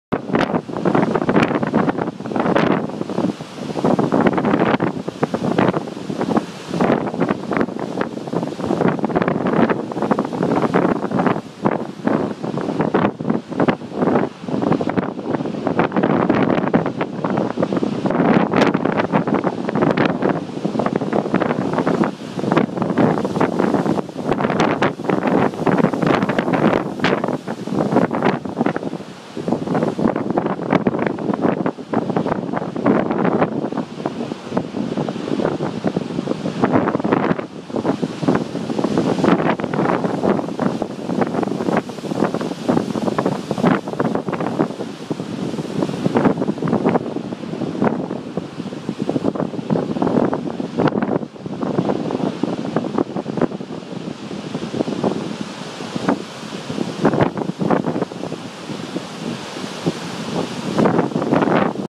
Kinard Swell 1/2/2014
The massive swell completely engulfed the sea stack when I stopped recording.